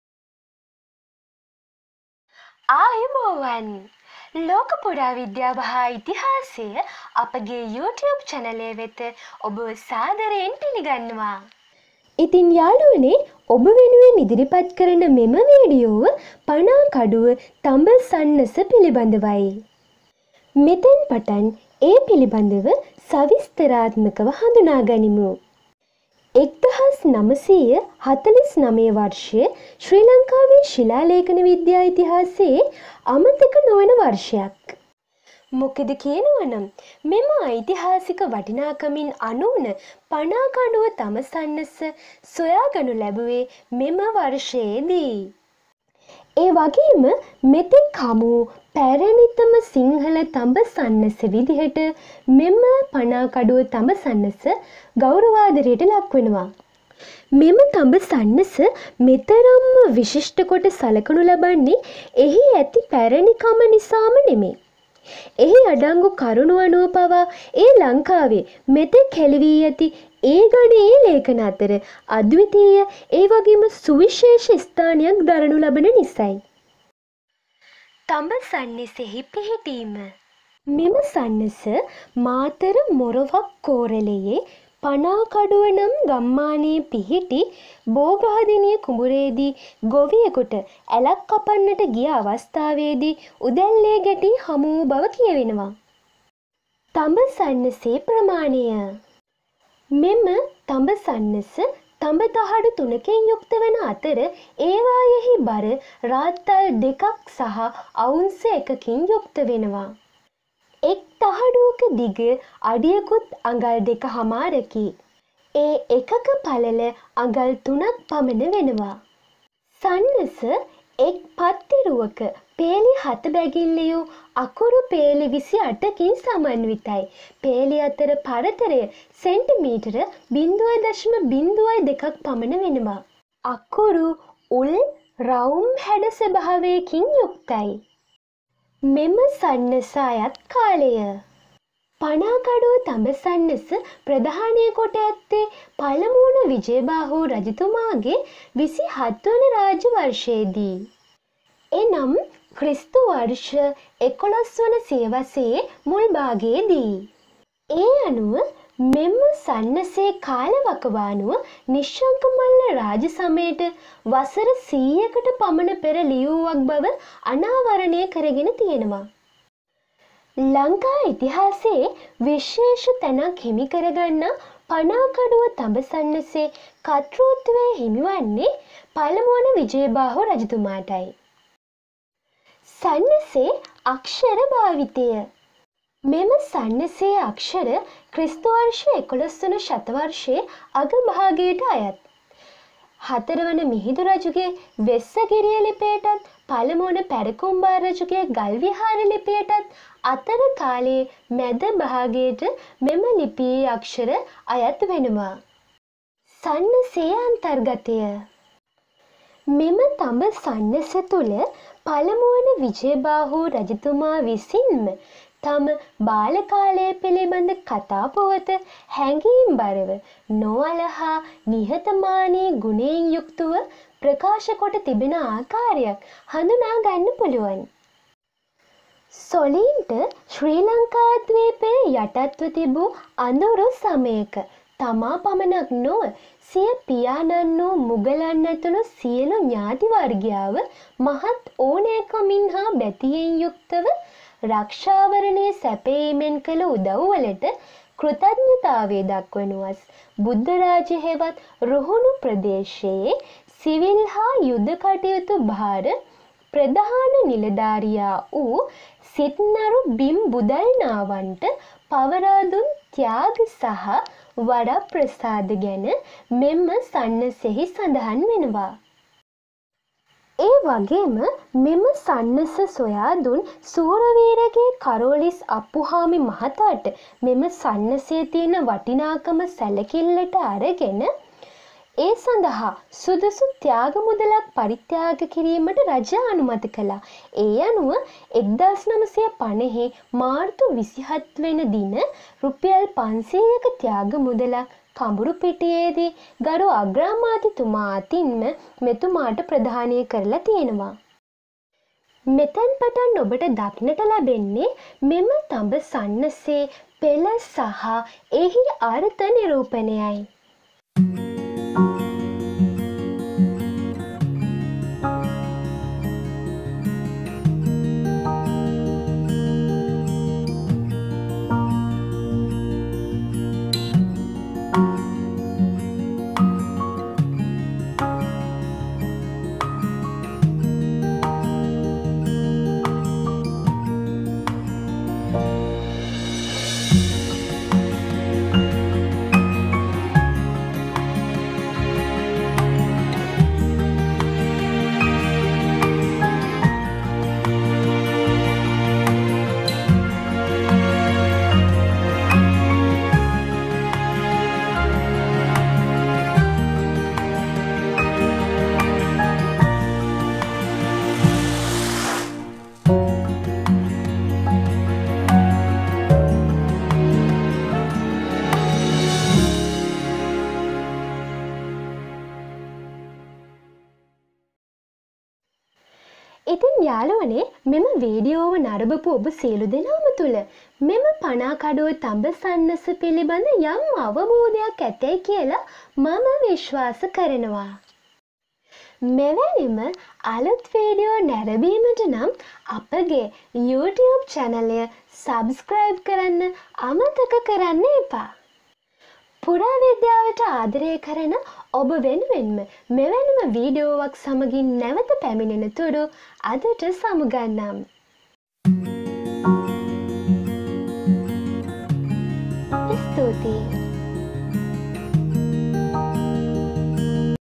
පනාකඩුව තඹ සන්නස(panakaduwa thaba sannasa)
පලමුවන විජයභාහු රජු විසින් නිකුත් කල සන්නස